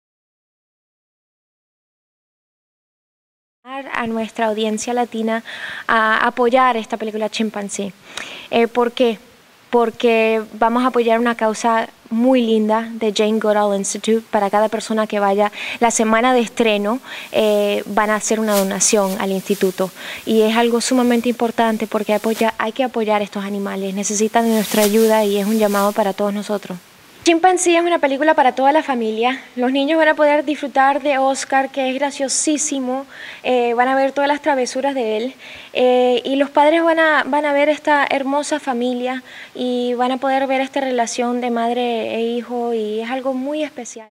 Génesis Rodríguez apoya la película "Chimpanzee" de Disneynature
Génesis Rodríguez habla sobre la importancia de apoyar esta cinta en el margen de la celebración del Día de la Tierra. La cinta se estrena el 20 de abril.